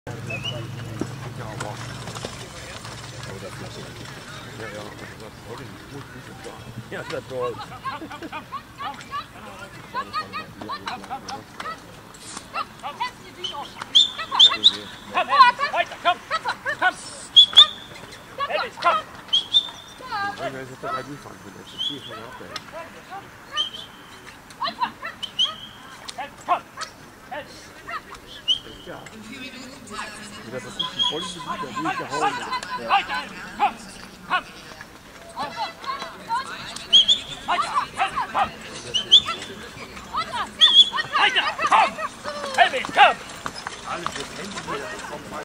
Fahrturnier Gronau/Epe 3.9.2023
Ein kurzes Video einer Marathon-Fahr-Prüfung vom Fahrturnier in Gronau/Epe
Deutsche Meisterschaft der Einspänner Fahrer
Münsterländer Mannschaftscup